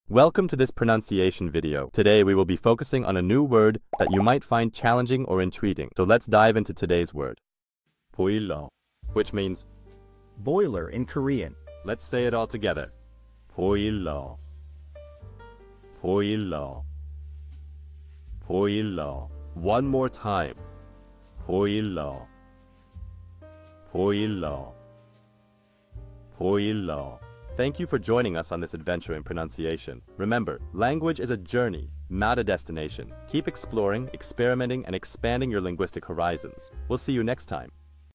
How to pronounce boilreo | 보일러 (Boiler in Korean)
Master the Pronunciation of 'boilreo | 보일러' - which means : Boiler in Korean 📝 with @PronunciationKorean 🎓🗣️ - [Your Guide to Korean]

Learn to pronounce 'boilreo' perfectly in Korean with our easy and engaging tutorial by @PronunciationKorean! 📖📽️ In this comprehensive video, we provide you with native speaker examples 🗣️👂 and phonetic breakdowns 🎵 to ensure you get the pronunciation just right.